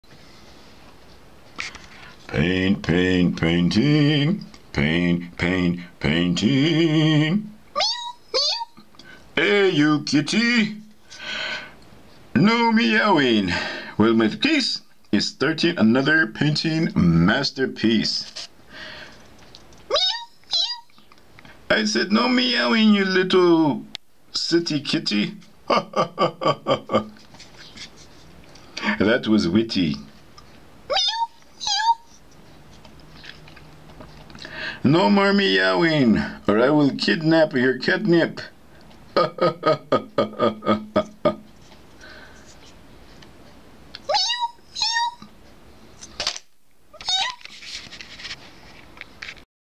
Starting A Painting - Masktisse
Masktisse lets us in on the starting process of a painting. Singing and a kitty too.